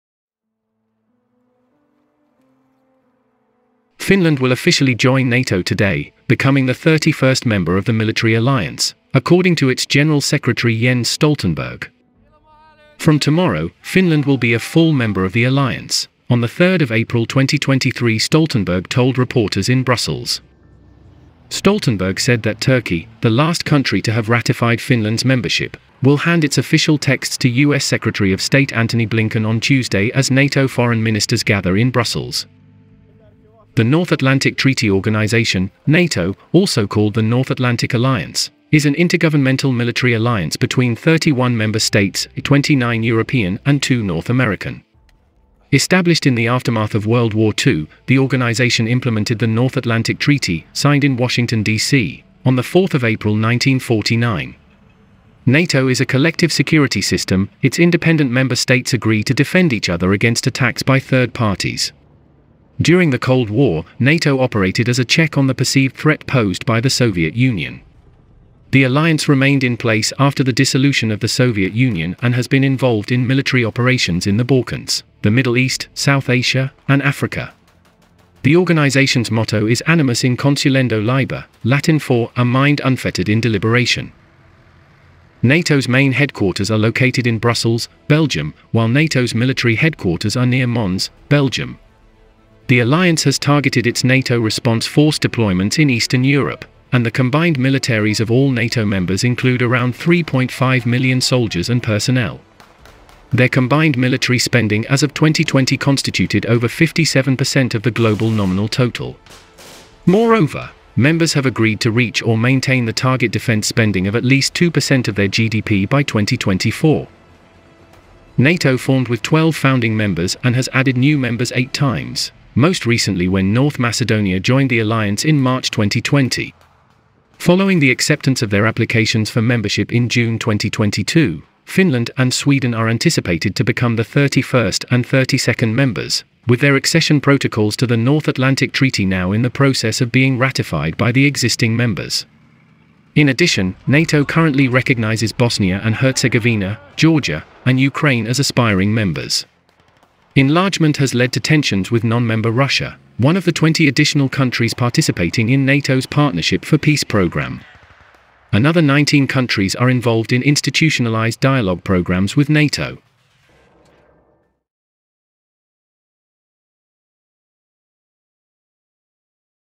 Finland will officially join NATO today! What is NATO? Military News Video
Finland will officially join NATO today, becoming the 31st member of the military alliance, according to its general secretary Jens Stoltenberg, Military News Video

'From tomorrow, Finland will be a full member of the alliance,' on the 3rd of APRIL 2023 Stoltenberg told reporters in Brussels. Stoltenberg said that Turkey, the last country to have ratified Finland's membership, will hand its official texts to U.S. Secretary of State Antony Blinken on Tuesday as NATO foreign ministers gather in Brussels.

Military News. Video not independently verify. For illustrative purposes only
Video Source: Puolustusvoimat - Försvarsmakten - The Finnish Defence Forces